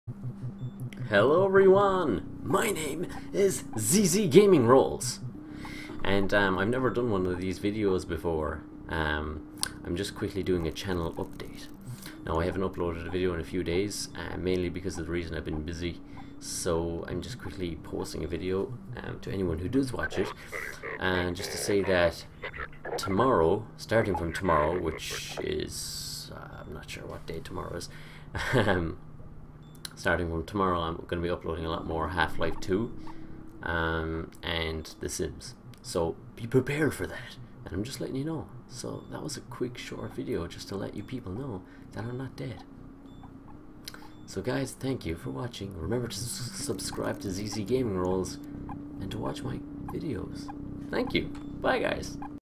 CHANNEL UPDATE: More Half-Life:2 Update / The Sims 3 to come!
Sorry haven't uploaded in 5 days, been busy, but starting from the 06/04/15, i will be uploading lots and lots of videos!